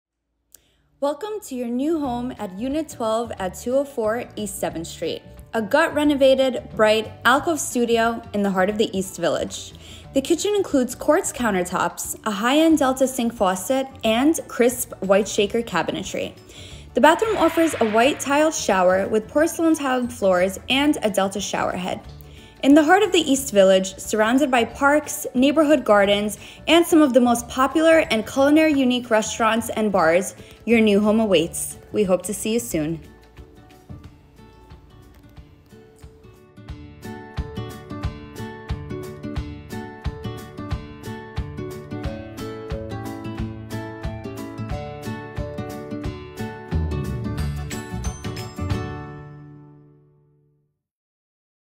Unit 12 at 204 East 7th Street - Sneak Peak Video
∗∗ East Village Charmer ∗∗ Fully Gut-Renovated, Bright Alcove Studio in Pet-Friendly Building on Tree-Lined Street, Steps from Huge Tompkins Square Dog Park ∗∗